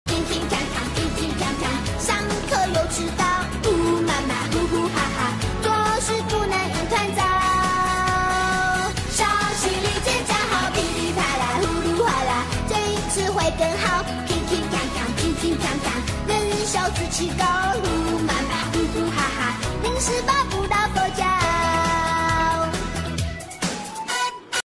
kids track train ride
Small track train ride shell with a new style of craft, each car has been crafted, with simulation of decorative pieces, like an art treasures. Coupled with the cheerful music, the atmosphere was very lively, distinctive theme. Popular by tourists, especially children's favorite, suitable for parks, indoor and outdoor playgrounds and other entertainment use.